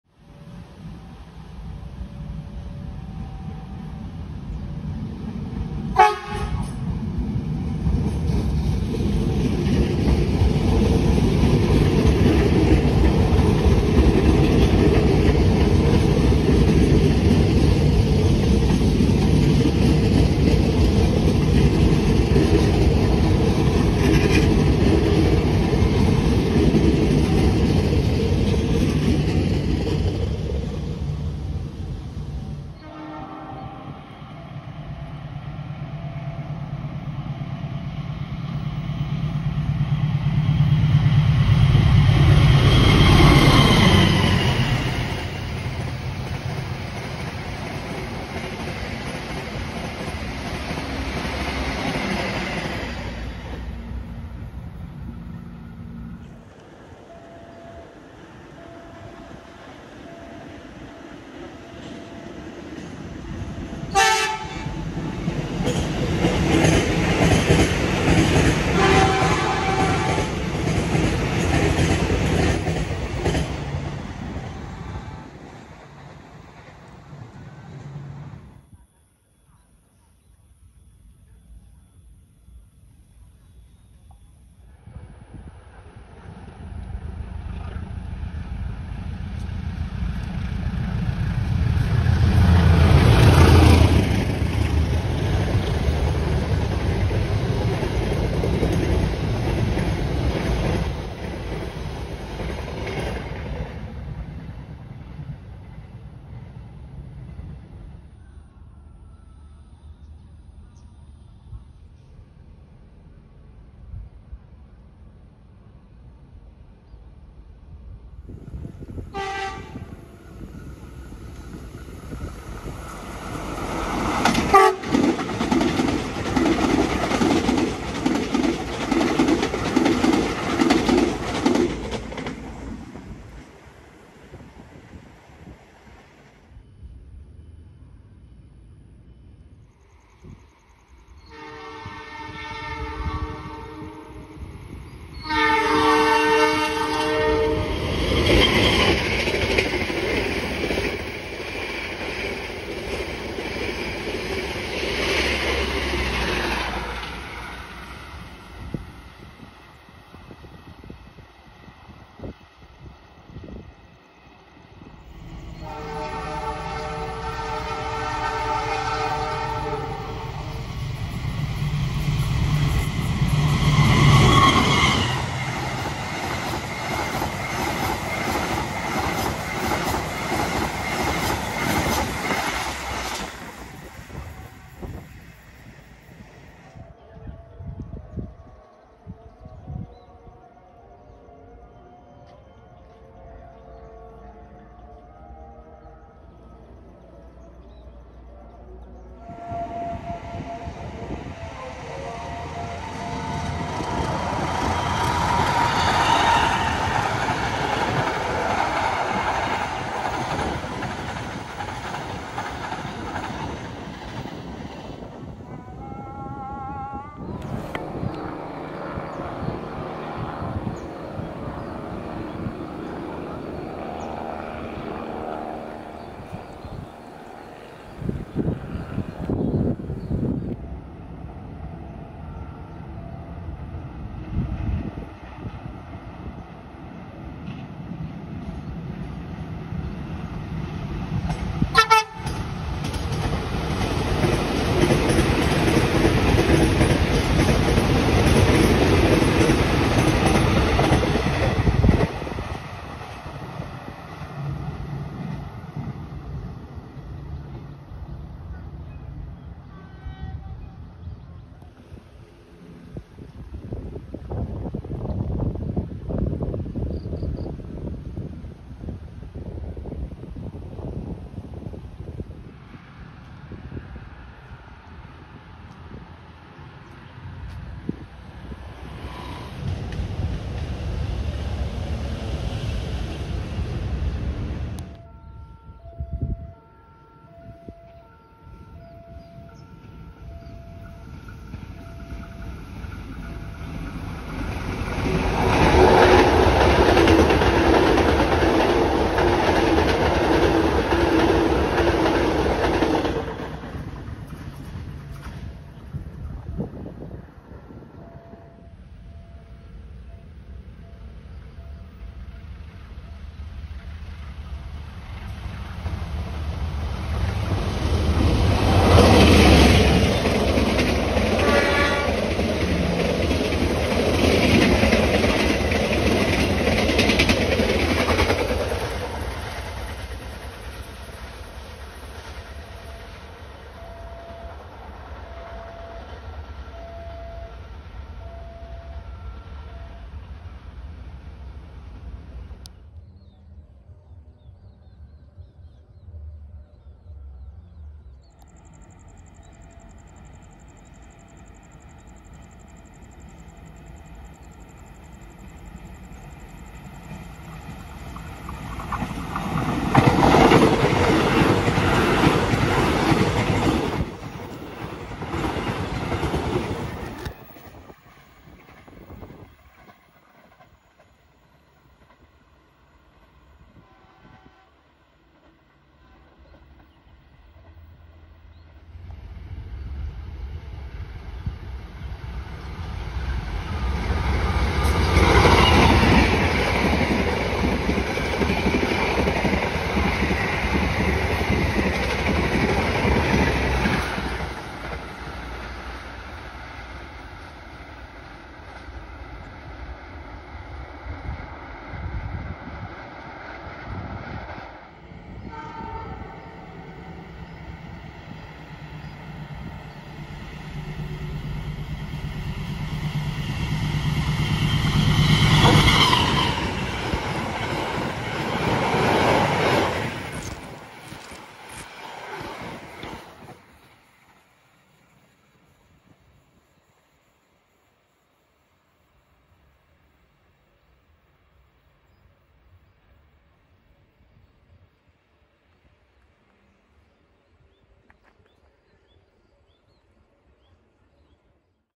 Trains at Kalasan
0:00 679 KRL for Yogyakarta
0:33 18 Argo Semeru for Surabaya Gubeng
0:56 680 KRL for Palur
1:25 92 Bengawan for Purwosari
1:58 673 KRL for Yogyakarta
2:25 99 Sancaka for Yogyakarta
2:45 10 Argo Dwipangga for Solo Balapan
3:19 162 Joglosemarkerto for Semarang Tawang
3:37 ?
3:50 672 KRL for Palur
4:45 681 KRL for Yogyakarta
5:09 211 Logawa for Purwokerto
5:48 682 KRL for Palur
6:18 105 Gaya Baru Malam Selatan for Pasar Senen
6:45 7014A Malabar for Malang